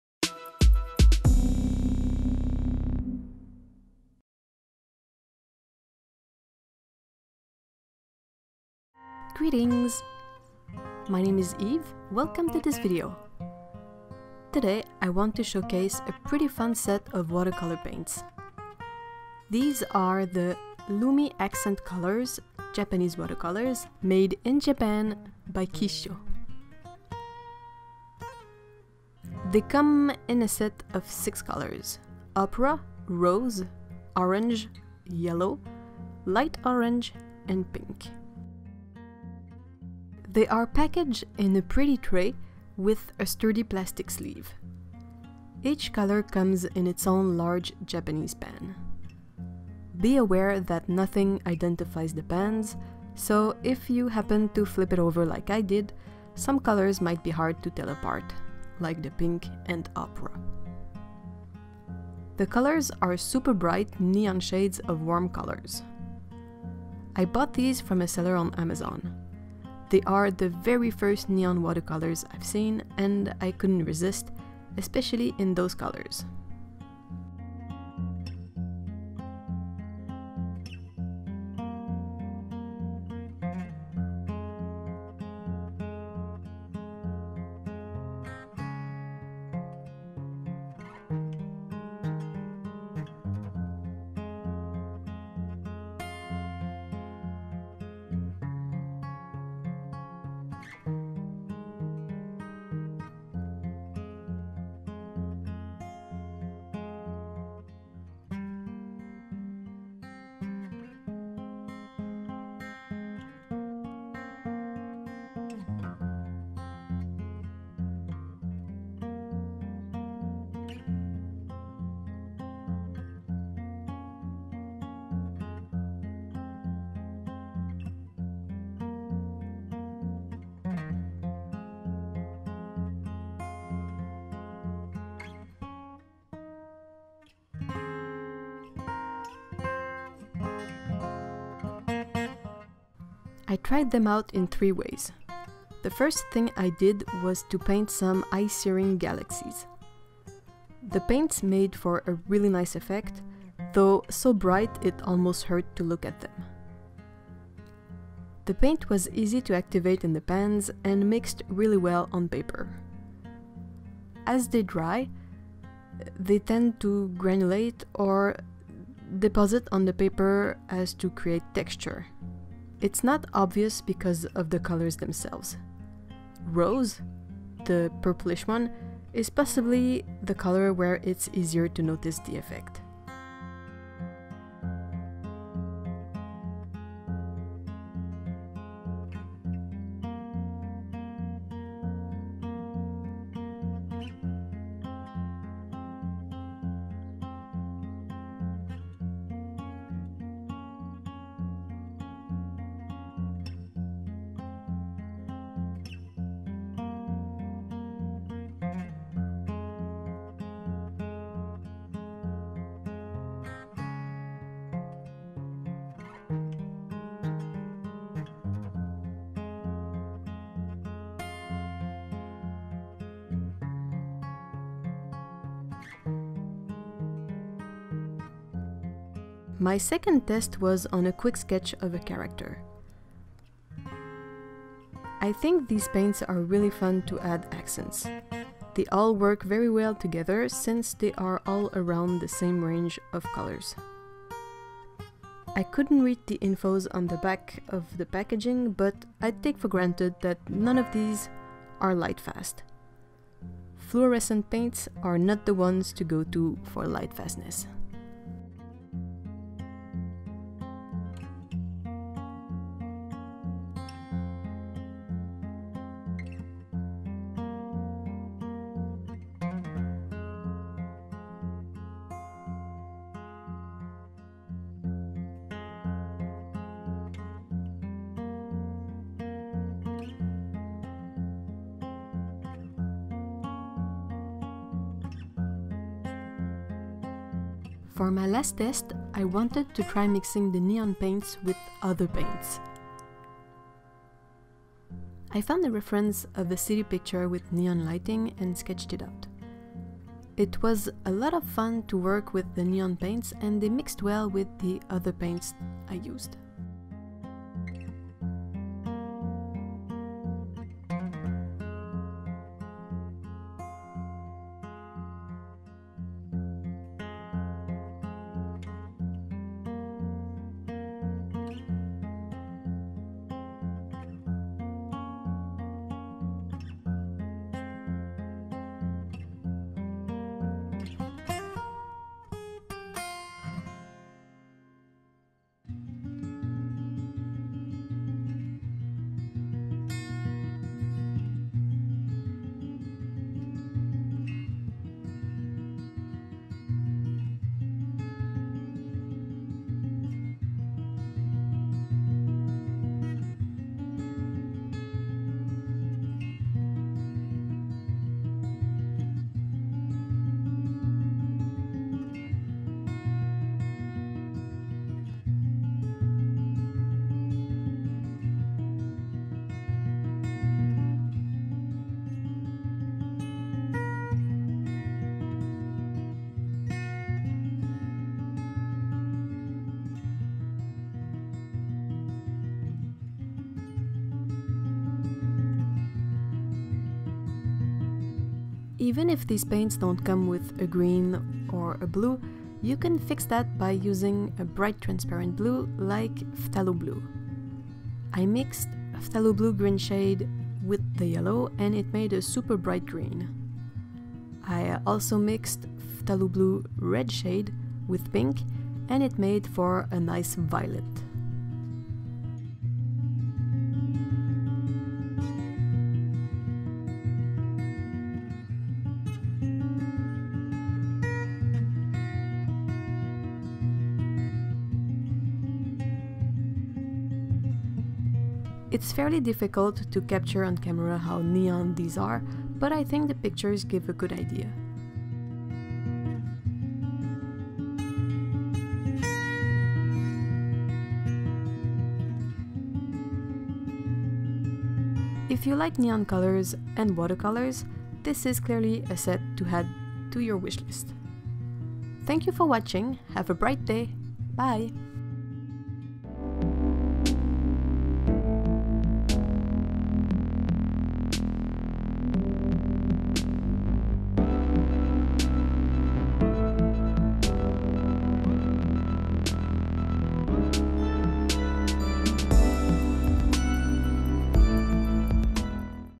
Review - Lumi Accent Color neon watercolors
Paints reviewed :
Kissho Lumi Accent Colors

Other colors used in the demos:
-Indanthrone blue, Perylene Violet and Maganese Blue Hue by Daniel Smith
-White Hydrus paint by Dr Ph Martin's

-Sketches were inked with a Carbon Platinum pen in extra-fine
-Sketchbook used is a Pentalic watercolor sketchbook
-I used Strathmore watercolor postcards for the galaxies